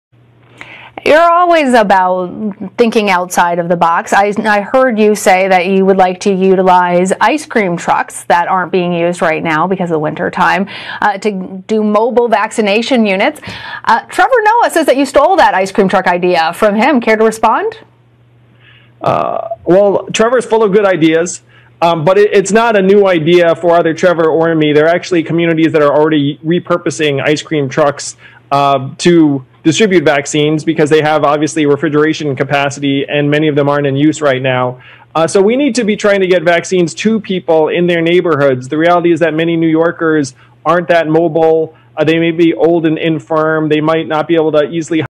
Andrew Yang vs Trevor Noah - Response to Stealing Ice Cream Truck Idea
Andrew Yang vs Trevor Noah 2 - Ice Cream Truck, the Response
Andrew Yang ice cream truck response to Trevor Noah